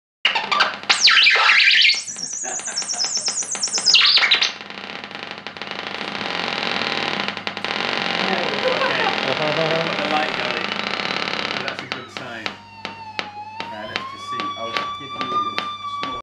Synth building workshop with The Noise Orchestra
Dec 2019
with The Spirit of Gravity at The Rose HIll